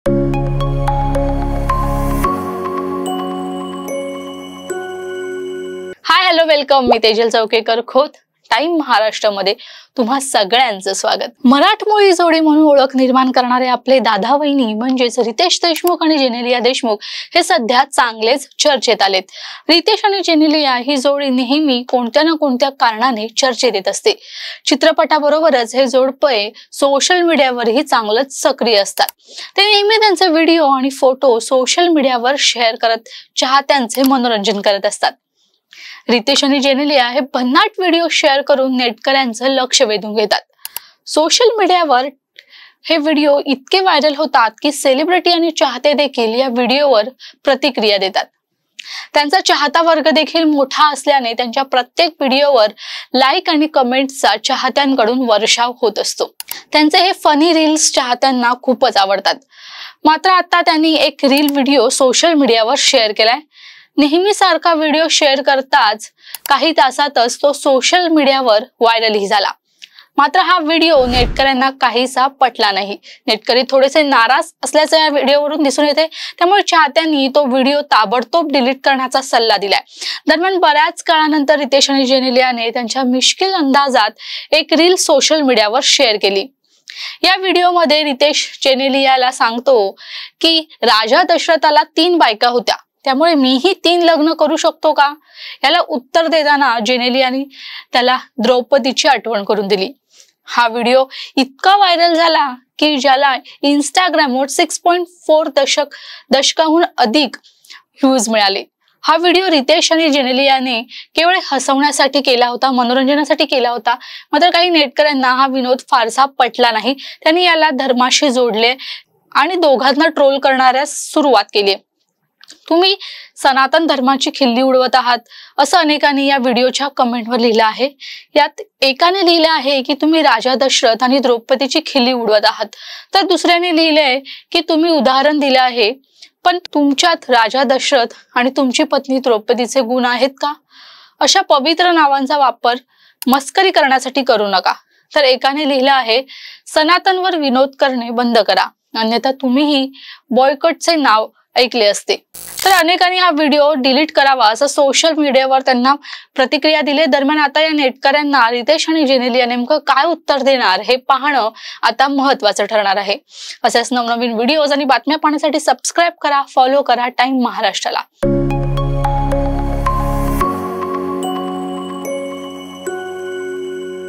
Riteish आणि Geneliaच्या व्हिडिओवर नेटकरी संतापले,रामायनाची थट्टा नाही | Riteish Deshmukh |Genelia
रितेश आणि जेनेलिया हे भन्नाट व्हिडिओ शेअर करुन नेटकऱ्यांचे लक्ष वेधून घेतात. सोशल मीडियावर हे व्हिडिओ इतके व्हायरल होतात की सेलिब्रेटी आणि चाहते देखील या व्हिडिओवर प्रतिक्रिया देतात.त्यांचा चाहता वर्ग देखील मोठा असल्याने त्यांच्या प्रत्येक व्हिडिओवर लाईक आणि कमेंट्सचा वर्षाव करत असतात.त्यांचे हे फनी रिल्स चाहत्यांना खुपच आवडतात.मात्र आता त्यांनी एक रिल व्हिडिओ सोशल मिडियावर शेयर केला.नेहमी सारखा व्हिडिओ शेअर करताच काही तासातच तो सोशल मिडियावर व्हायरलही झाला. मात्र हा व्हिडिओ नेटकऱ्यांना काहीसा पटला नाही.